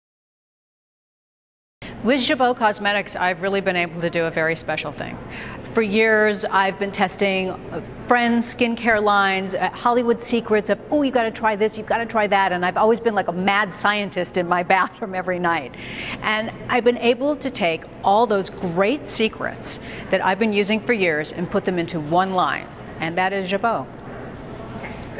Tracey Bregman Chats about Jabot Cosmetics Background Secrets!
Discover Tracey Bregman's inspiration for the Jabot Cosmetics line!